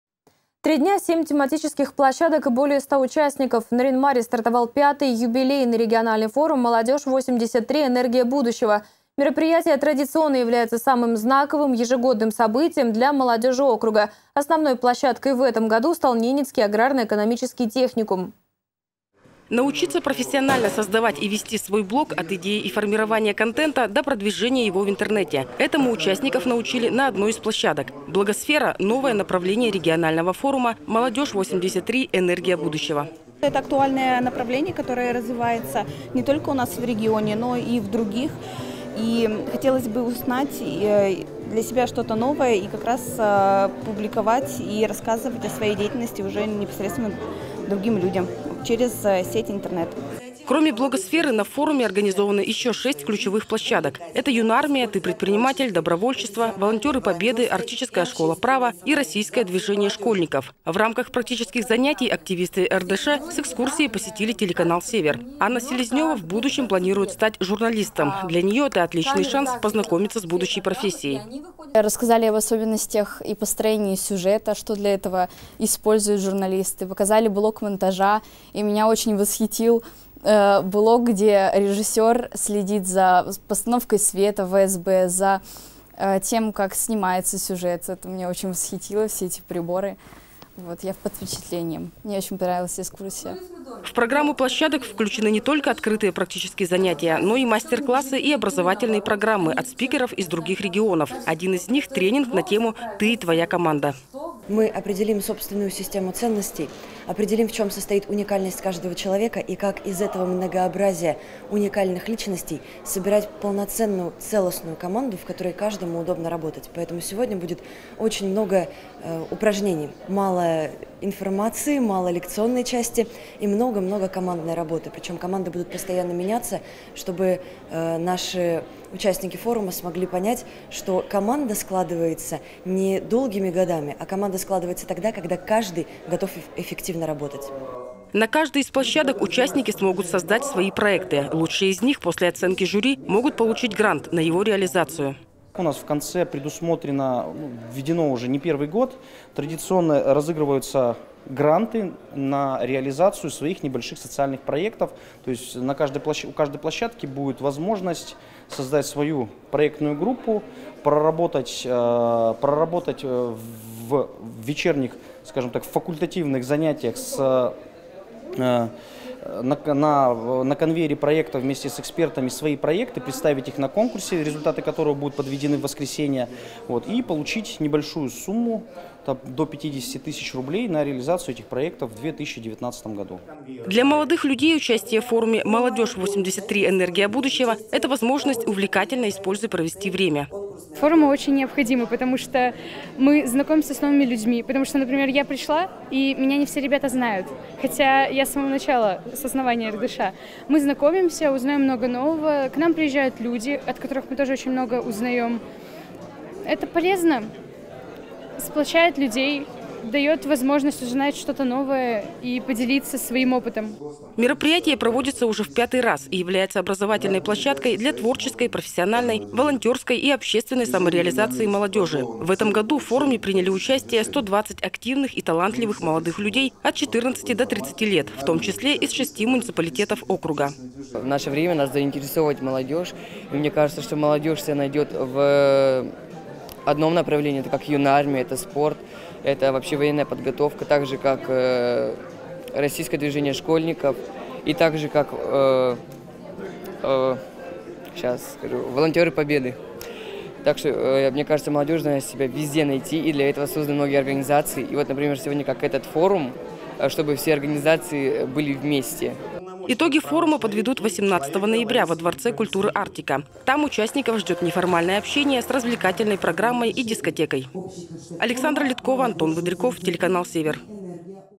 В Ненецком округе начал работу образовательный форум «Молодёжь 83 – Энергия округа!»
Три дня, семь тематических площадок и более ста участников. В Нарьян–Маре стартовал пятый юбилейный региональный форум «Молодежь 83 – Энергия округа!». Мероприятие считается самым знаковым событием года для окружной молодежи. Основной площадкой в этом году стал Ненецкий аграрно–экономический техникум.